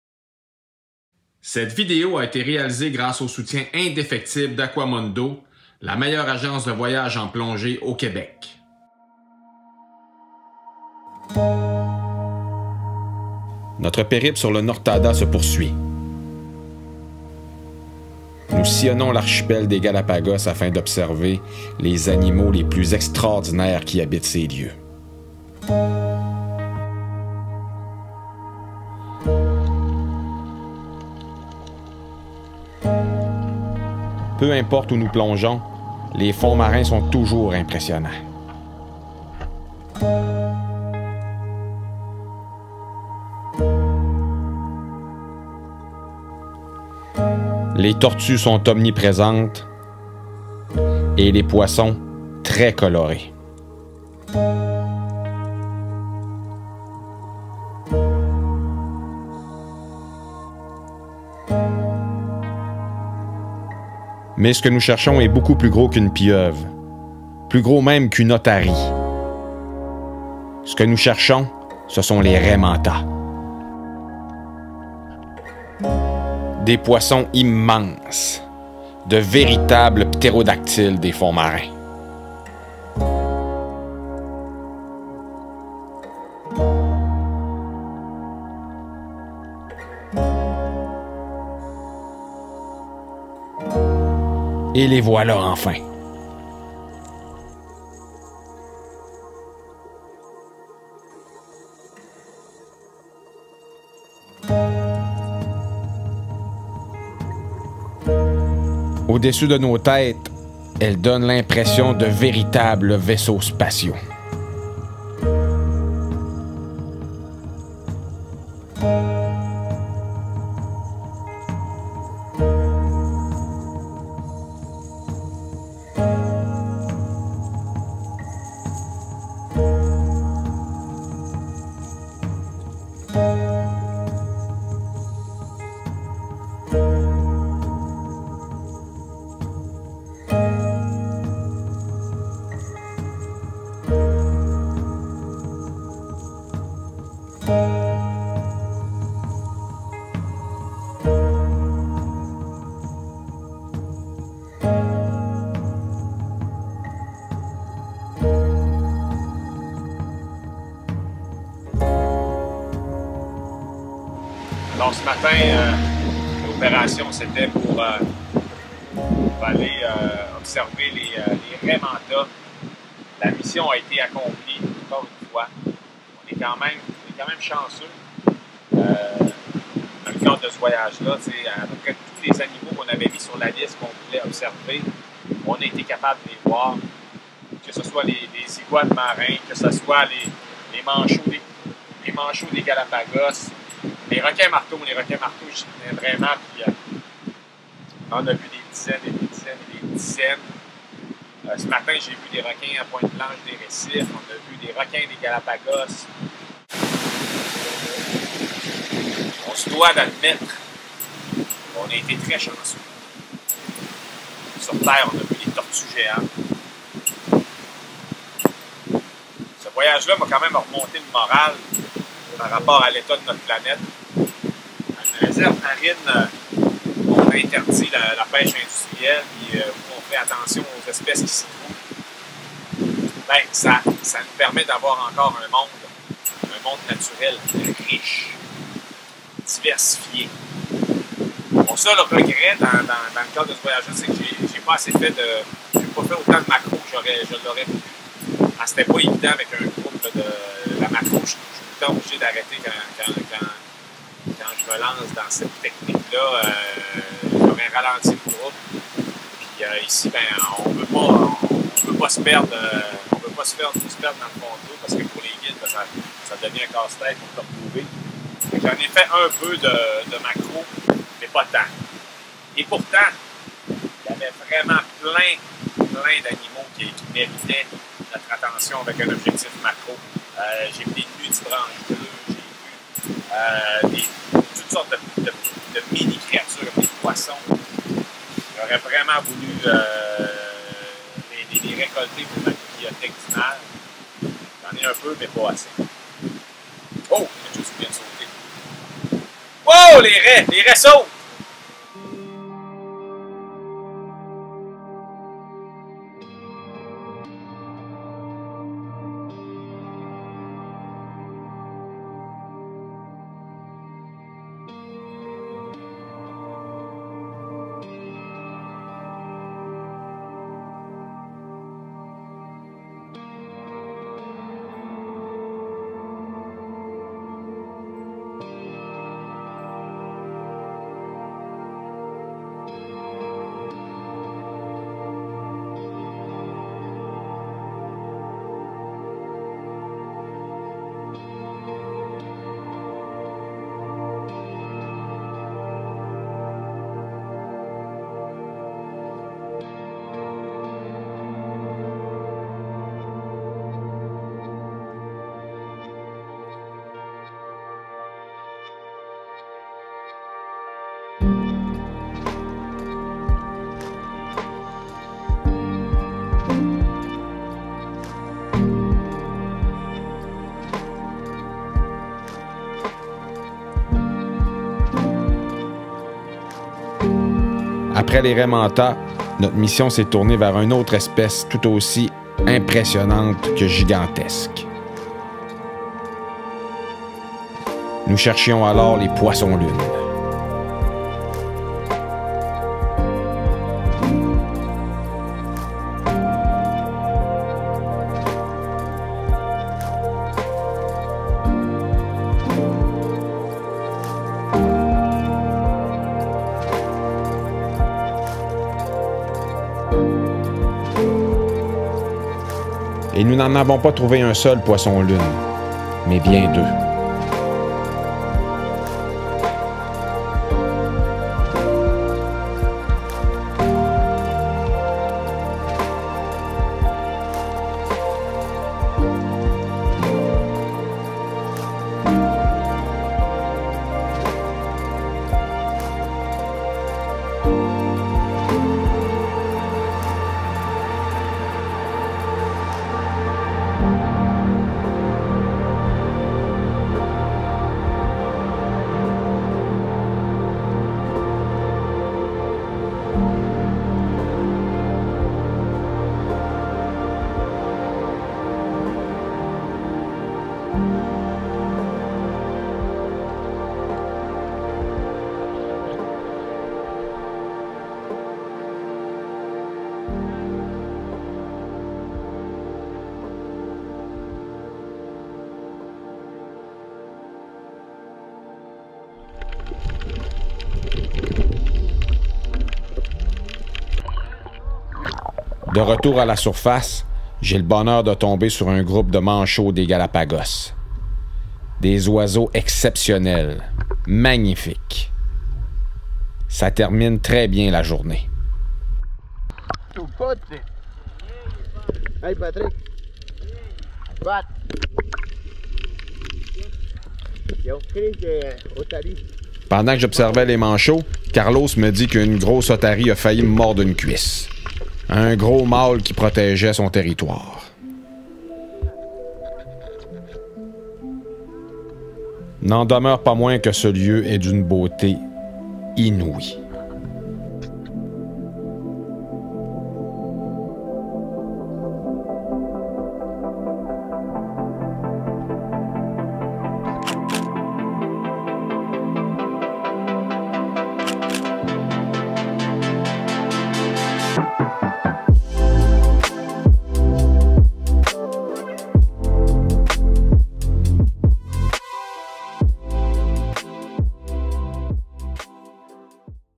[Photo sous-marine] La raie manta et le poisson-lune (Galapagos, 5e épisode)
Dans cette vidéo, je pars à la recherche de poissons qui figurent parmi les plus gros de nos océans : la raie manta et le poisson-lune.

Il s'agit du 5e épisode de mon aventure de plongée sous-marine aux Galapagos.